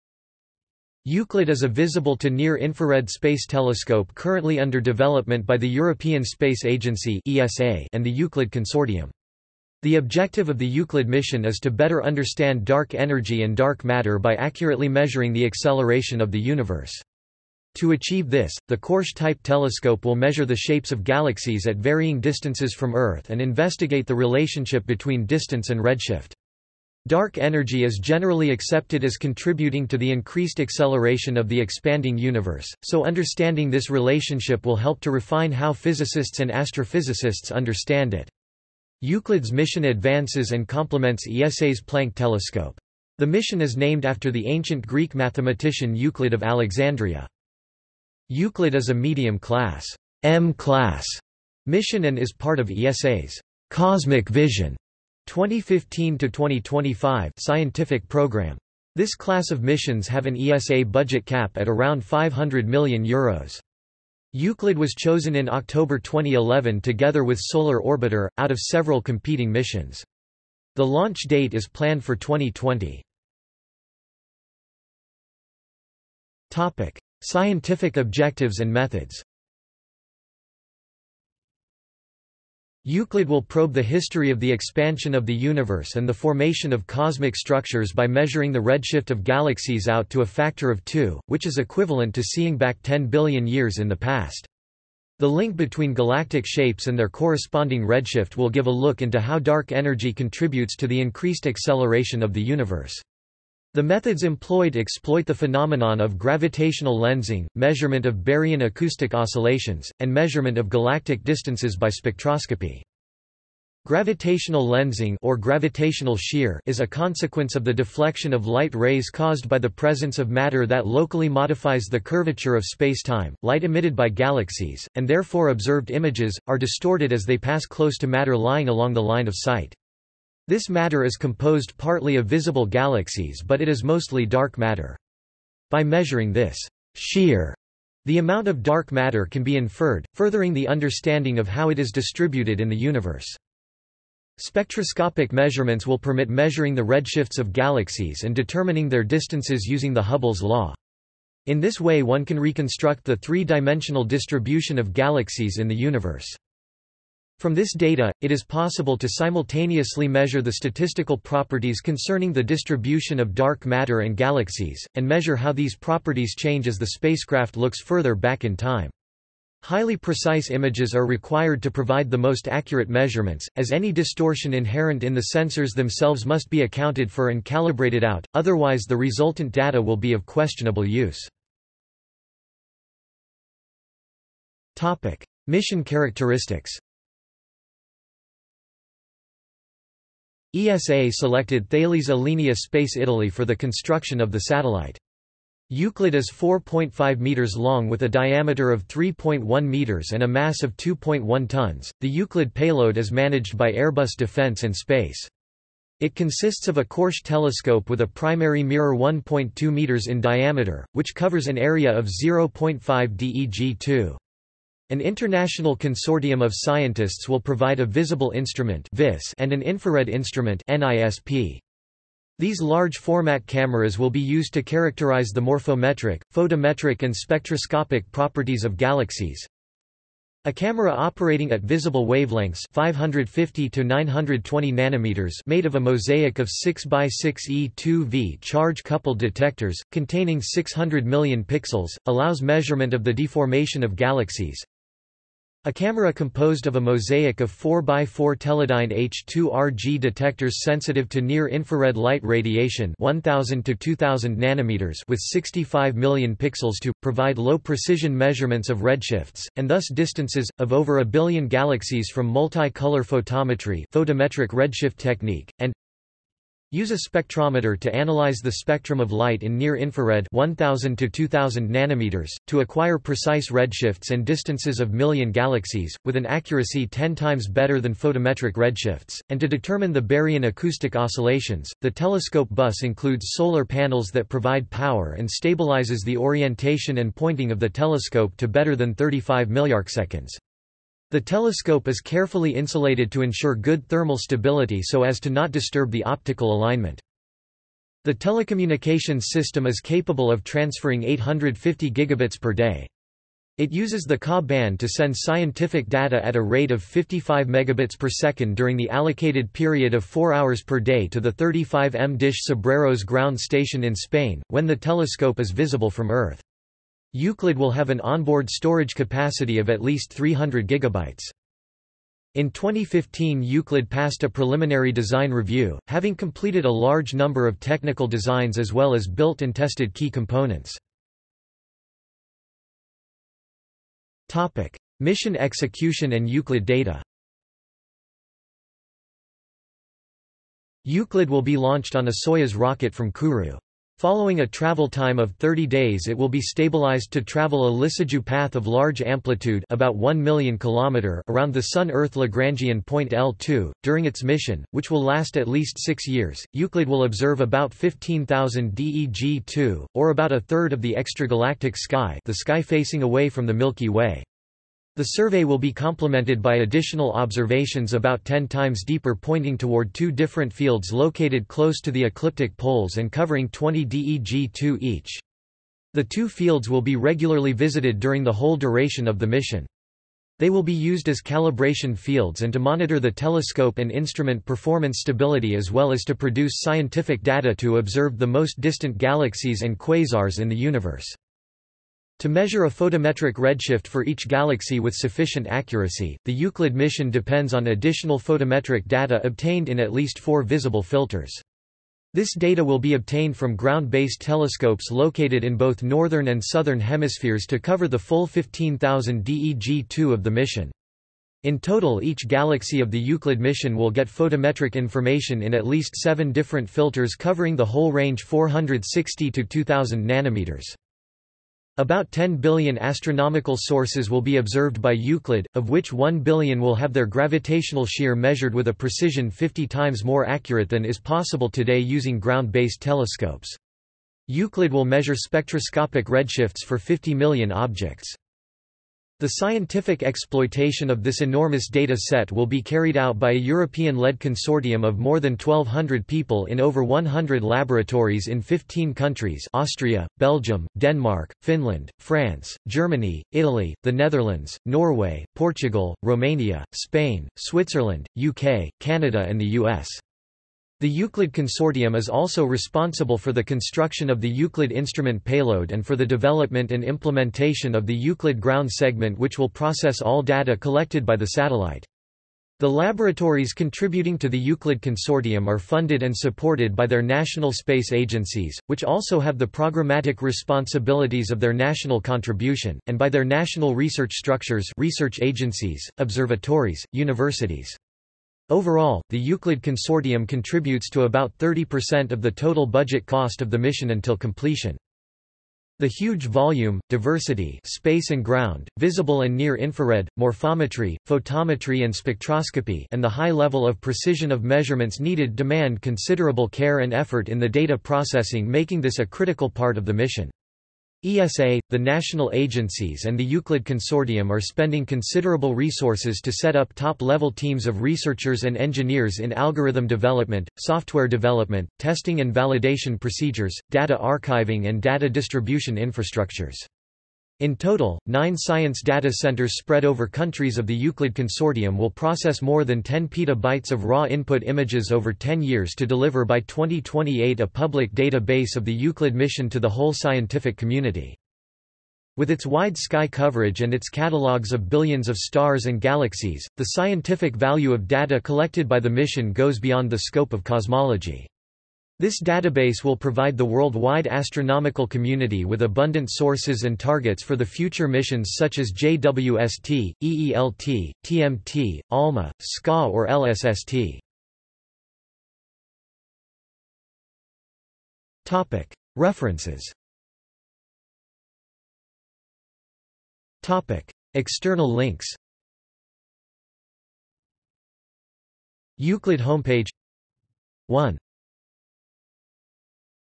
Euclid (spacecraft) | Wikipedia audio article
This is an audio version of the Wikipedia Article:
Euclid (spacecraft)

- Socrates

SUMMARY
Euclid is a visible to near-infrared space telescope currently under development by the European Space Agency (ESA) and the Euclid Consortium. The objective of the Euclid mission is to better understand dark energy and dark matter by accurately measuring the acceleration of the universe. To achieve this, the Korsch-type telescope will measure the shapes of galaxies at varying distances from Earth and investigate the relationship between distance and redshift. Dark energy is generally accepted as contributing to the increased acceleration of the expanding universe, so understanding this relationship will help to refine how physicists and astrophysicists understand it. Euclid's mission advances and complements ESA's Planck telescope.  The mission is named after the ancient Greek mathematician Euclid of Alexandria.
Euclid is a medium-class ("M-class") mission and is part of ESA's "Cosmic Vision" (2015–2025) scientific program. This class of missions have an ESA budget cap at around €500 million. Euclid was chosen in October 2011 together with Solar Orbiter, out of several competing missions. The launch date is planned for 2020.